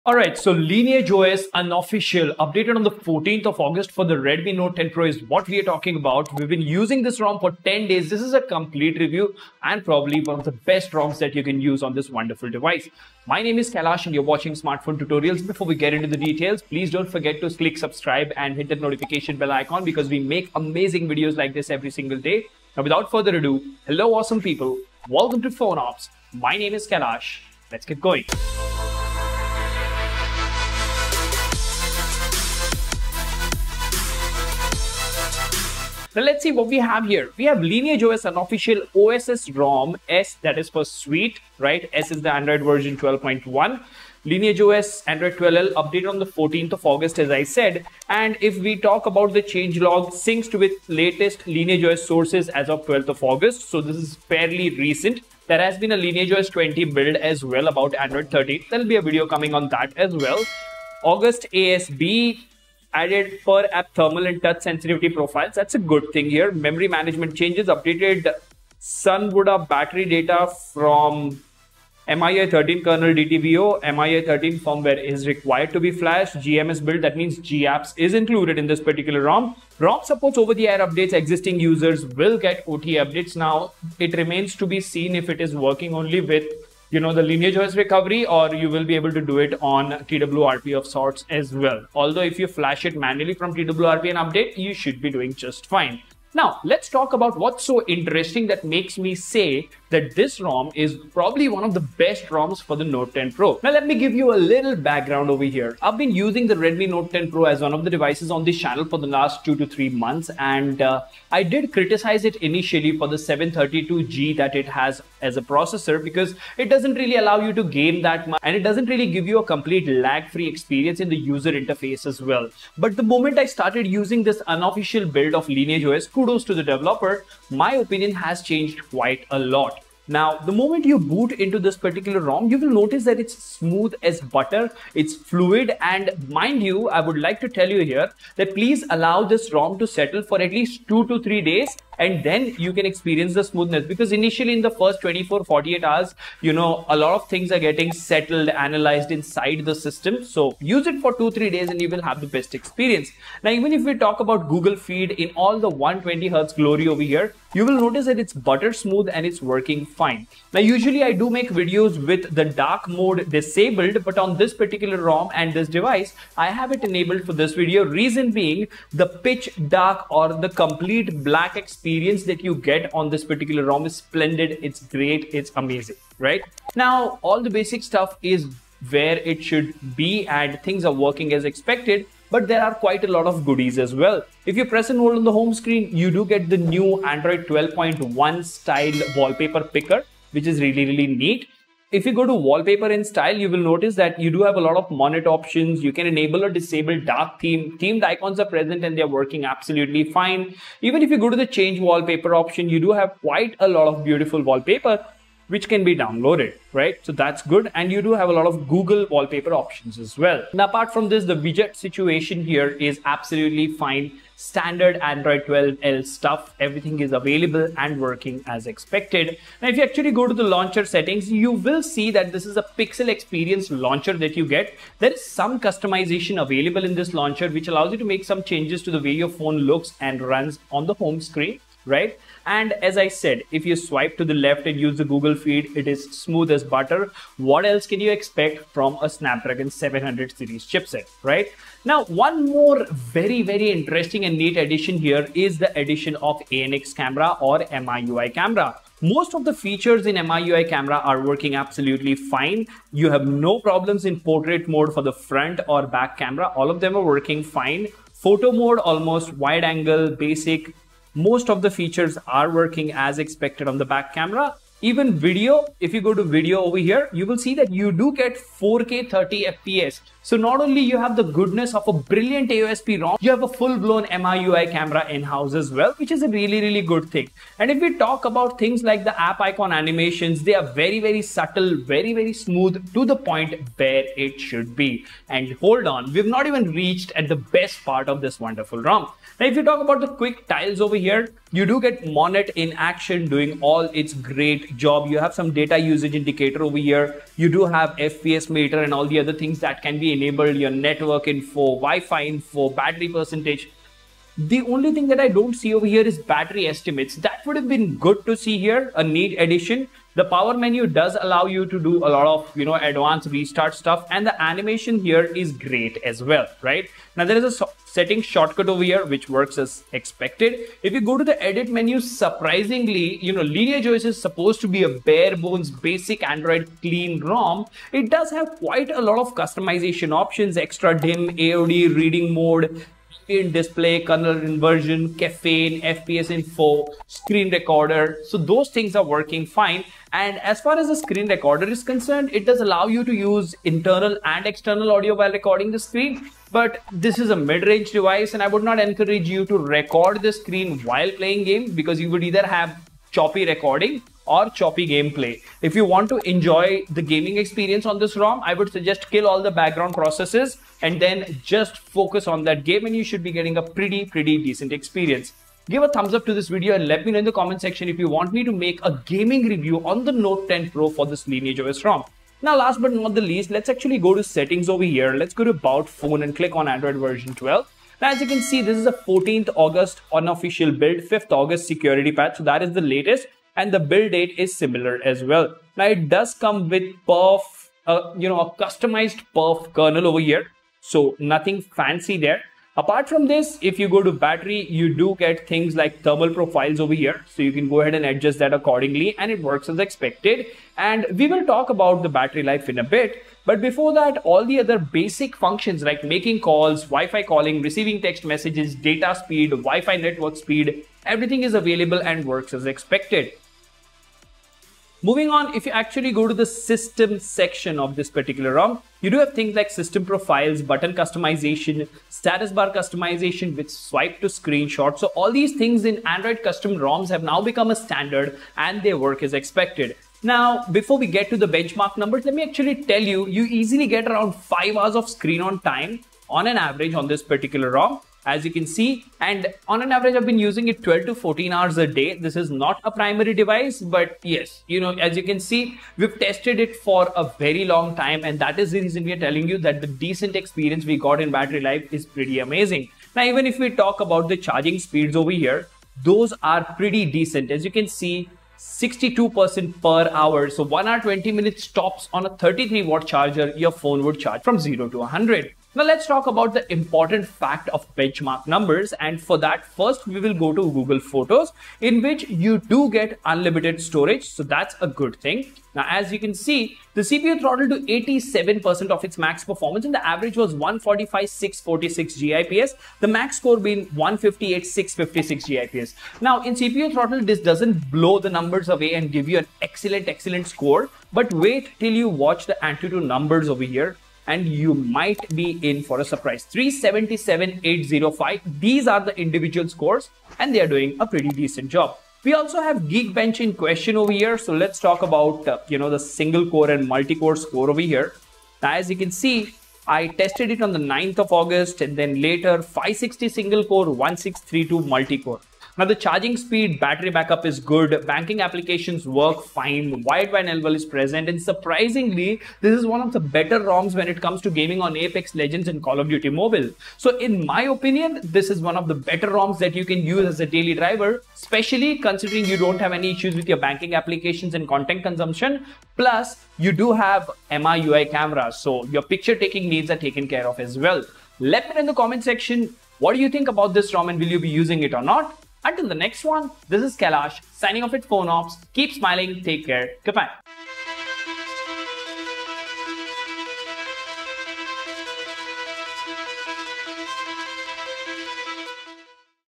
Note 10 Pro + Lineage Os 19.1 : Amazing All-round Performance
Whats the best custom rom for Redmi Note 10 Pro. Watch the complete review to know more.